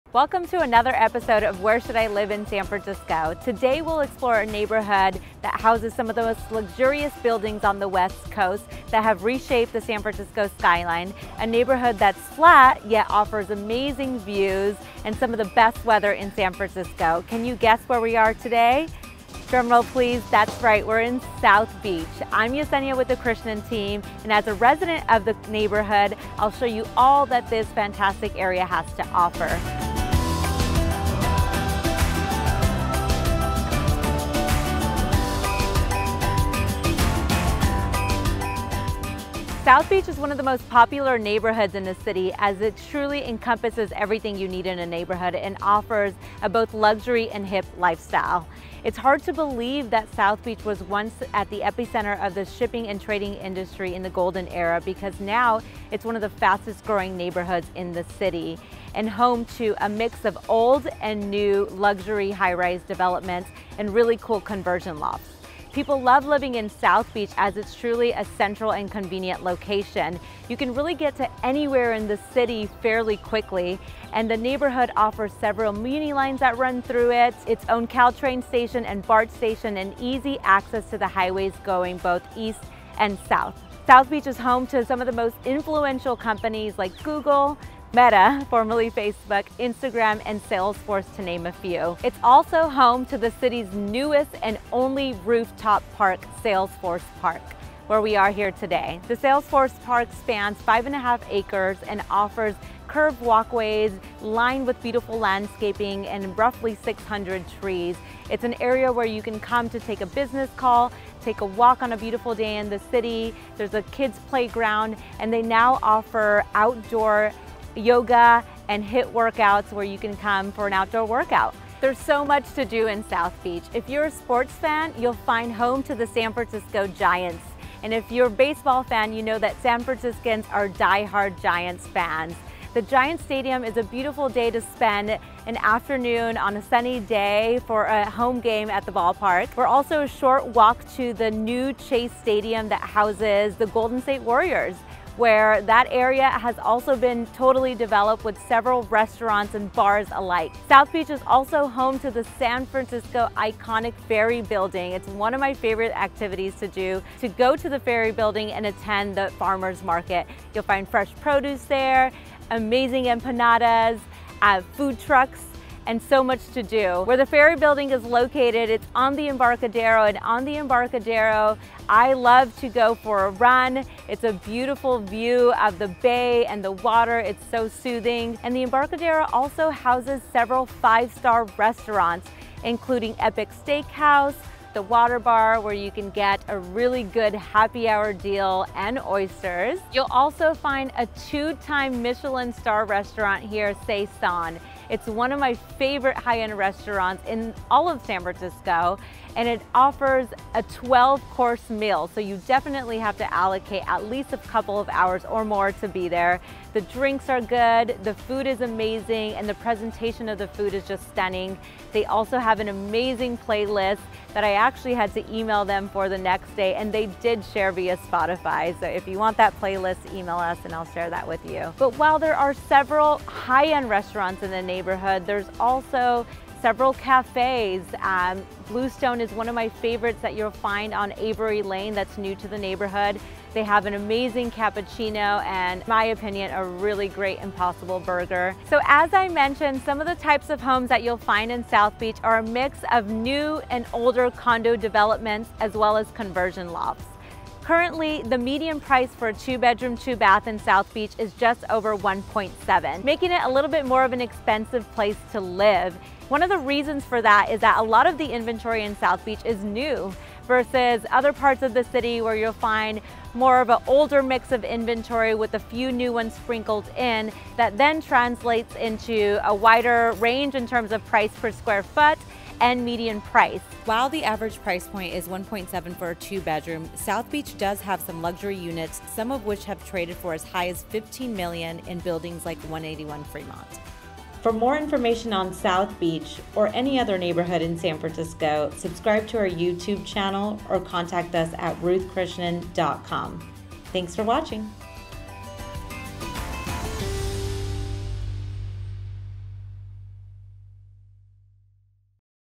Where to live in San Francisco: All about the South Beach neighborhood, real estate and more
Learn about living in South Beach, San Francisco neighborhood real estate, home sales and price data, local features, and more. We'll explore a neighborhood that houses some of the most luxurious buildings on the west coast that have reshaped the San Francisco skyline.

A neighborhood that's flat yet offers amazing views and some of the best weather in San Francisco. Can you guess where we are today? Drumroll please. That's right. We're in South Beach. I'm Yesenia with The Krishnan Team and as a resident of the neighborhood and I'll show you all that this fantastic area has to offer.

South Beach is one of the most popular neighborhoods in this city as it truly encompasses everything you need in a neighborhood and offers a both luxury and hip lifestyle. It's hard to believe that South Beach was once at the epicenter of the shipping and trading industry in the Golden Era because now it's one of the fastest growing neighborhoods in the city and home to a mix of old and new luxury high rise developments and really cool conversion lofts.

People love living in South Beach as it's truly a central and convenient location. You can really get to anywhere in the city fairly quickly. And the neighborhood offers several Muni lines that run through it its own Caltrain station and Bart station and easy access to the highways going both East and South.

South Beach is home to some of the most influential companies like Google Meta, formerly Facebook, Instagram and Salesforce to name a few. It's also home to the city's newest and only rooftop park. Salesforce Park where we are here today. The Salesforce Park spans 5.5 acres and offers curved walkways lined with beautiful landscaping and roughly 600 trees. It's an area where you can come to take a business call take a walk on a beautiful day in the city. There's a kids' playground and they now offer outdoor yoga and HIIT workouts where you can come for an outdoor workout.

There's so much to do in South Beach. If you're a sports fan, you'll find home to the San Francisco Giants. And if you're a baseball fan you know that San Franciscans are die-hard Giants fans. The Giants stadium is a beautiful day to spend an afternoon on a sunny day for a home game at the ballpark. We're also a short walk to the new Chase stadium that houses the Golden State Warriors where that area has also been totally developed with several restaurants and bars alike. South Beach is also home to the iconic San Francisco Ferry Building. It's one of my favorite activities to do to go to the ferry building and attend the farmer's market. You'll find fresh produce there, amazing empanadas food trucks and so much to do.

Where the Ferry Building is located it's on the Embarcadero and on the Embarcadero I love to go for a run. It's a beautiful view of the Bay and the water. It's so soothing. And the Embarcadero also houses several 5 star restaurants including Epic steakhouse, The WaterBar where you can get a really good happy hour deal and oysters. You'll also find a two time Michelin star restaurant here Saison. It's one of my favorite high end restaurants in all of San Francisco and it offers a 12 course meal so you definitely have to allocate at least a couple of hours or more to be there. The drinks are good, the food is amazing and the presentation of the food is just stunning. They also have an amazing playlist that I actually had to email them for the next day and they did share via Spotify.

But while there are several high end restaurants in the neighborhood there's also several cafes. Bluestone is one of my favorites that you'll find on Avery Lane that's new to the neighborhood. They have an amazing cappuccino and in my opinion a really great Impossible burger.

So as I mentioned some of the types of homes that you'll find in South Beach are a mix of new and older condo developments as well as conversion lofts. Currently the median price for a 2 bedroom 2 bath in South Beach is just over $1.7 million making it a little bit more of an expensive place to live. One of the reasons for that is that a lot of the inventory in South Beach is new versus other parts of the city where you'll find more of a older mix of inventory with a few new ones sprinkled in that then translates into a wider range in terms of price-per-square foot and median price. While the average price point is $1.7 million for a 2 bedroom South Beach does have some luxury units some of which have traded for as high as $15 million in buildings like 181 Fremont.

Compass DRE# 01862279